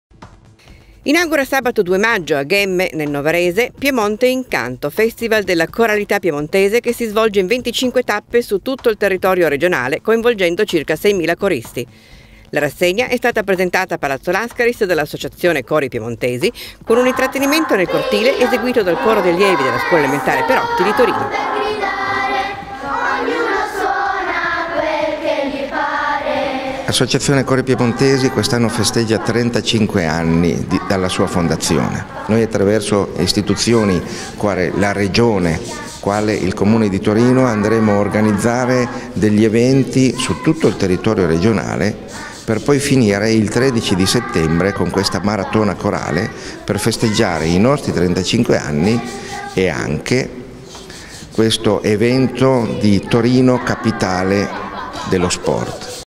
Cori a Palazzo Lascaris
Inaugura sabato 2 maggio a Ghemme, nel novarese, “Piemonte in… canto”, festival della coralità piemontese che si svolge in 25 tappe su tutto il territorio regionale, coinvolgendo circa seimila coristi . La rassegna è stata presentata a Palazzo Lascaris dall’Associazione Cori piemontesi, con un intrattenimento nel cortile eseguito dagli allievi della scuola elementare Perotti di Torino.
[intervista a Ettore Galvani, presidente dell’Associazione Cori piemontesi]